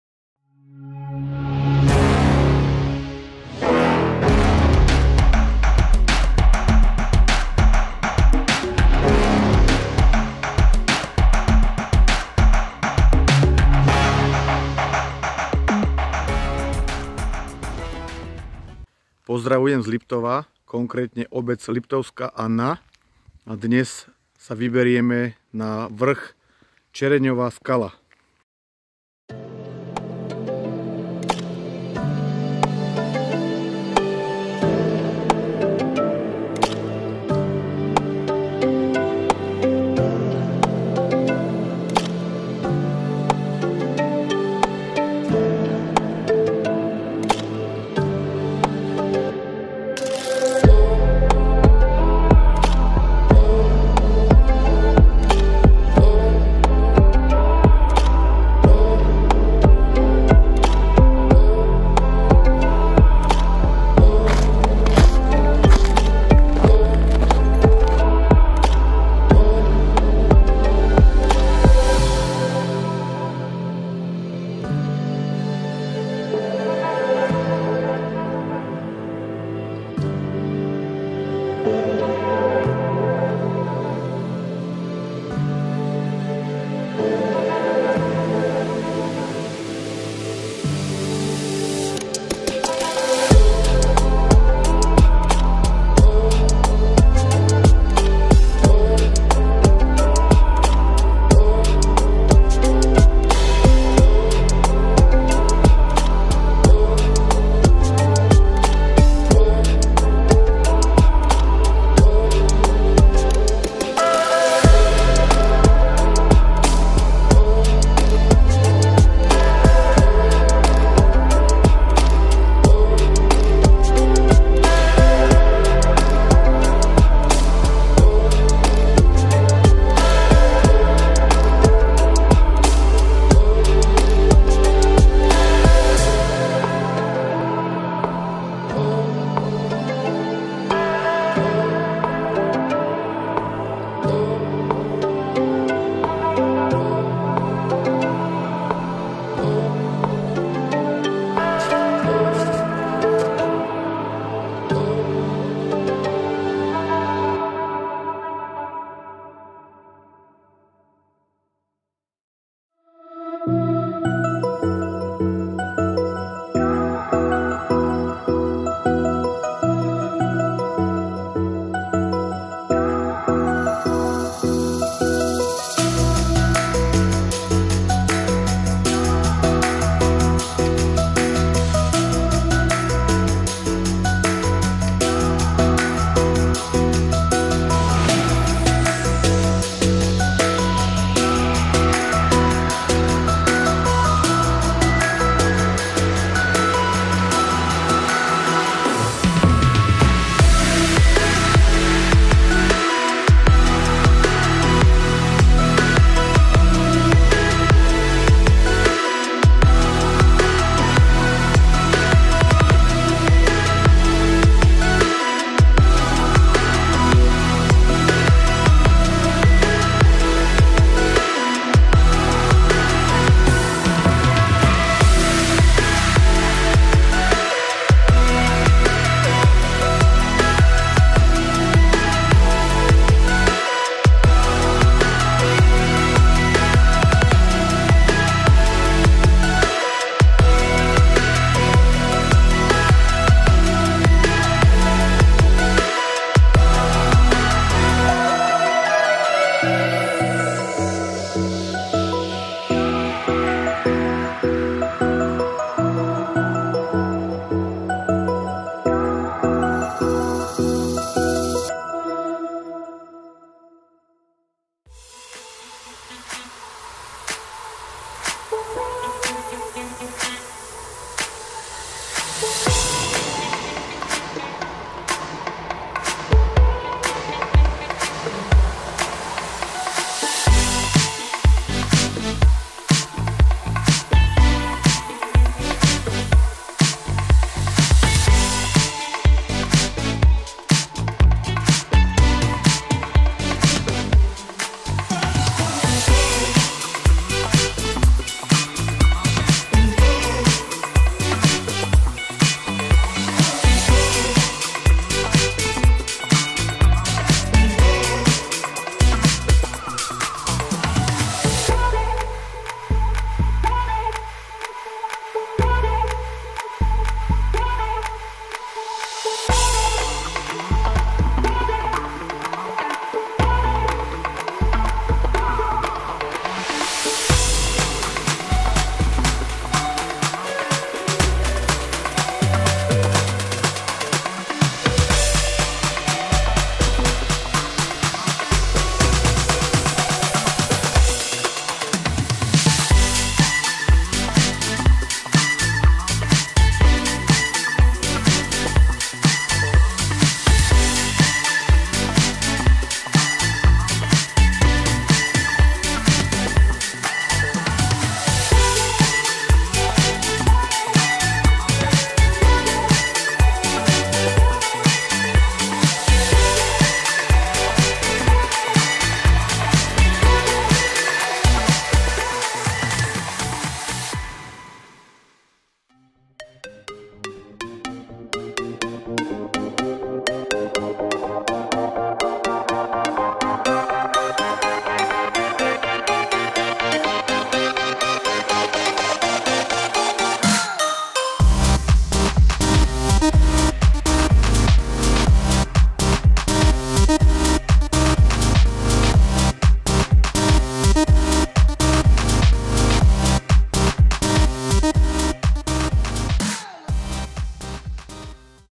Liptovská Anna-Čereňová skala a nakoniec Kalameny: KÚPAČKA😎19.12.2023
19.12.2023 výstup z obce Liptovská Anna na Bačov stolec alebo aj Černová skala poskytla nám nádherné výhľady na západné Tatry Liptovsku Maru ,Nízke Tatry a okolité kopce a na záver kúpacka v neďalekých Kalamenoch 👌👍